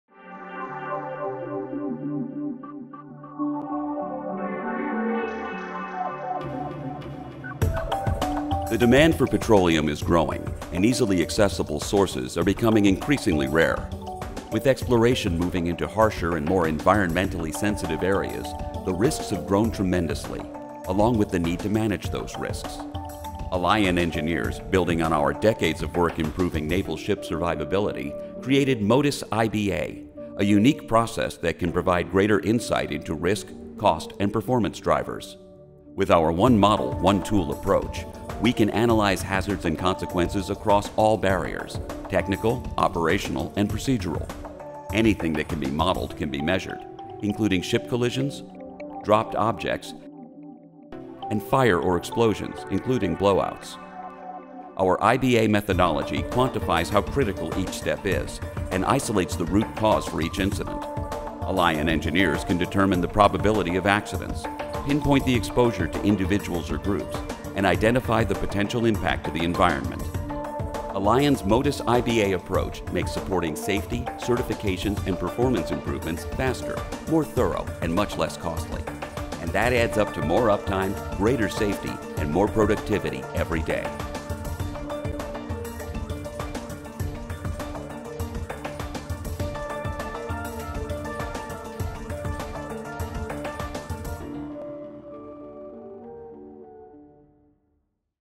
Alion Offshore Risk, Safety and Uptime Solutions
Operating offshore drilling and production platforms is inherently risky. Now, Alion Offshore makes identifying and managing risks easier, faster and significantly less costly with our Measure of Total Integrated System Survivability - Integrated Barrier Analysis (MOTISS-IBA™), a unique process that can identify vulnerabilities and help improve uptime.